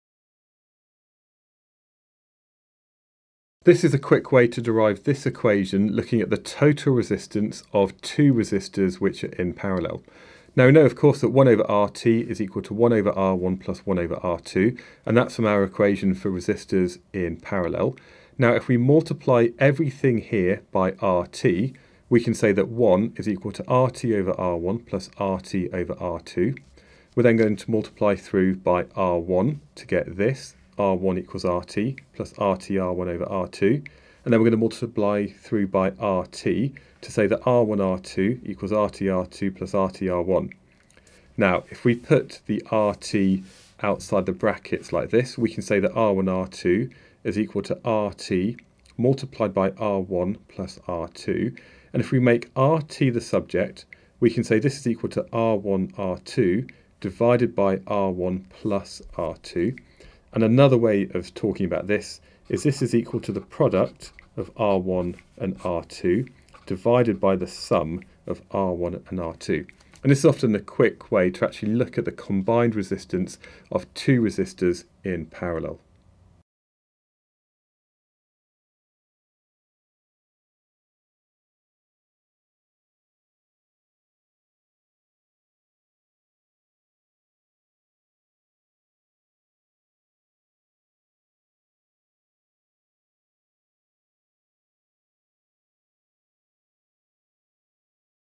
How to Derive Two Resistors in Parallel Quickly - A Level Physics
How to quickly derive the equation for two resistors in parallel for A Level Physics.

Download the revision sheet (and other similar worksheets) for this quick Physics derivation and many more A Level Physics derivations at: